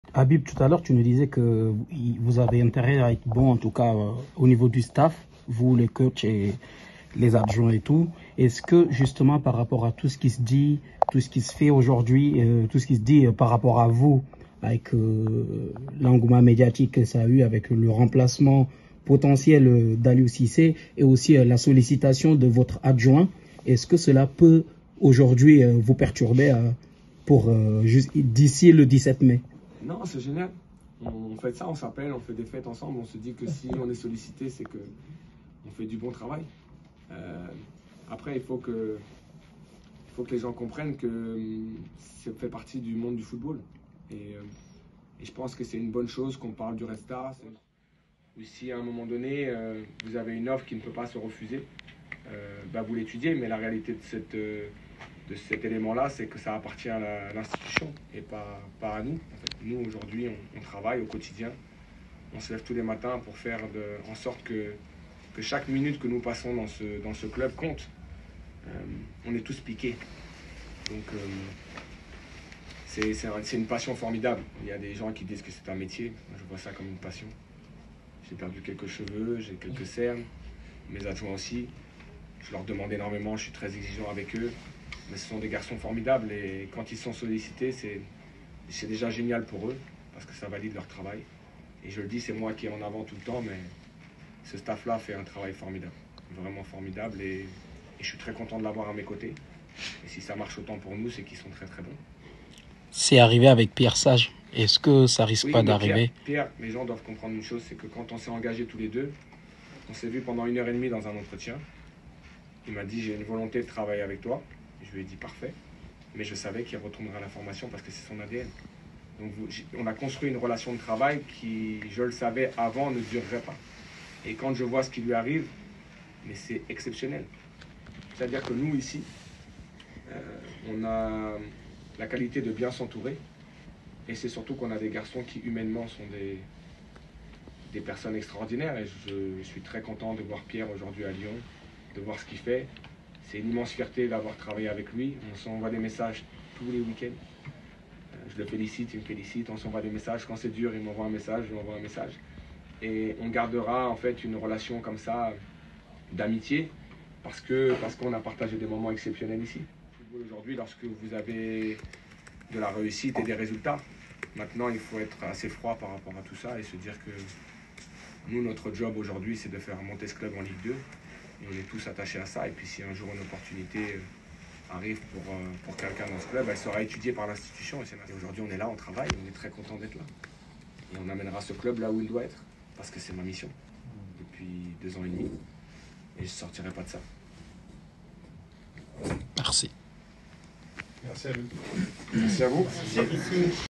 Habib Beye se prononce sur les sollicitations venant de tout bord
En conférence de presse d’après match,Habib Beye l’entraîneur du Red Star ,parle des sollicitations et des rumeurs ,pour un poste de sélectionneur de l’équipe nationale du Sénégal